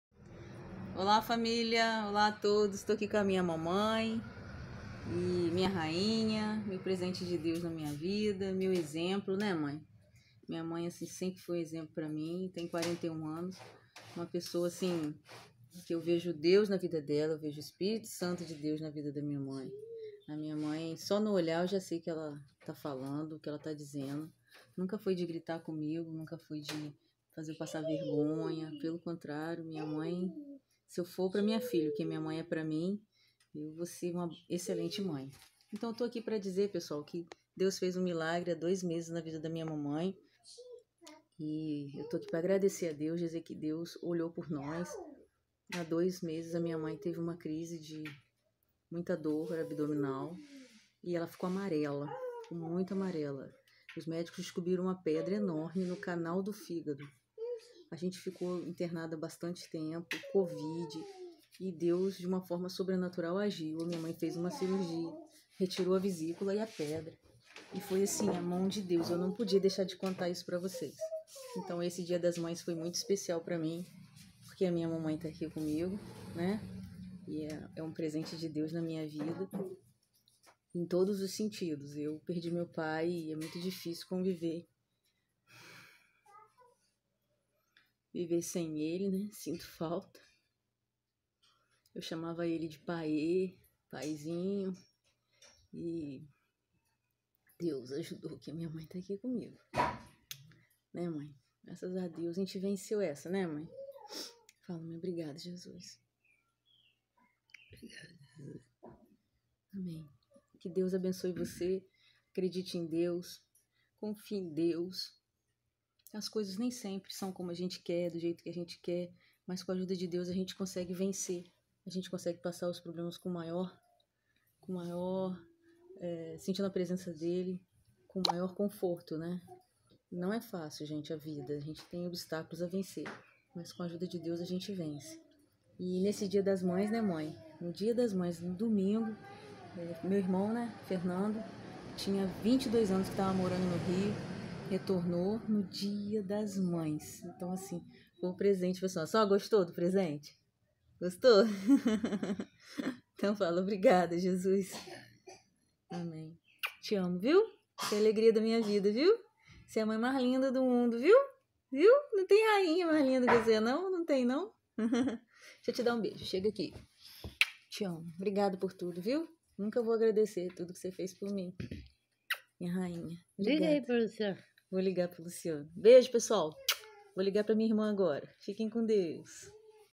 Testemunho: Minha mãe, um milagre de Deus! Obrigada Jesus.
Glórias a Deus! Mãe, um milagre de Deus! Obrigada Jesus por ter cuidado da minha rainha.
Passamos meses apreensivos, minha mãe estava amarela e com dores e não foi rápido o diagnóstico. Enfim, os médicos descobriram uma pedra enorme no canal do fígado. Minha Mãezinha passou por uma cirurgia, e para glória de Deus estamos agradecendo a Deus pelo livramento.
Obrigada Jesus, por sua presença, por seus cuidados. Obrigada Jesus por tudo! O que seria de mim se não fosse a sua presença santa que acalma? O que seria de nós se não fosse a sua mão conosco? Não posso realizar um culto de ações de graças, mas quero pagar meu voto e publicar para todos os meus amigos saberem o quanto eu dependo de Ti, óh meu Deus!